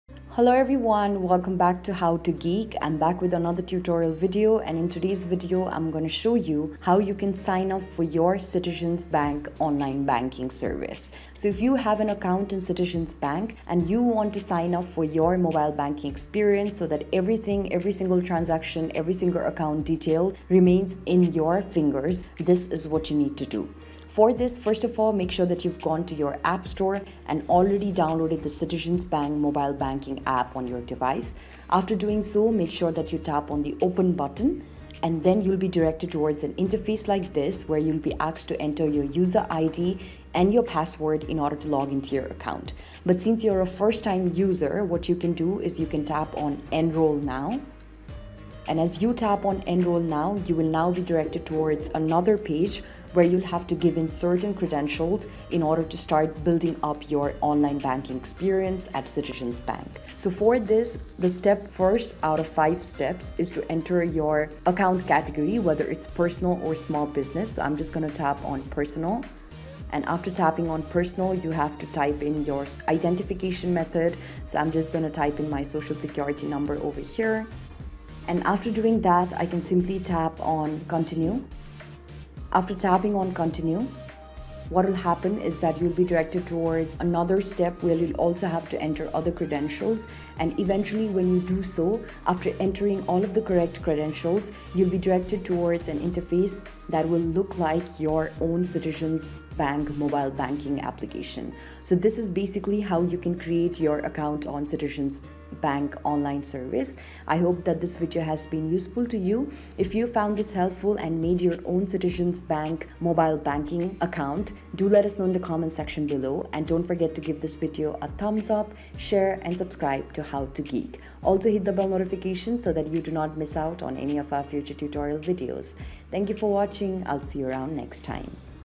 Enroll in Online Banking - Citizens Bank - Mobile Banking Sign Up
Want to sign up to the online banking of Citizen Bank but don’t know how? Well, you’ve landed on just the right video.
Go ahead and follow the steps listed down below:
1. Firstly, go to the play store and search for Citizens Bank Mobile Banking.
2. Once the app is displayed, tap on the get or install icon besides it to download it.
4. Select the type of account and desired identification number.
5. Enter the selected identification number and tap on continue.
6. Fill out the personal details as mandated to complete the registration.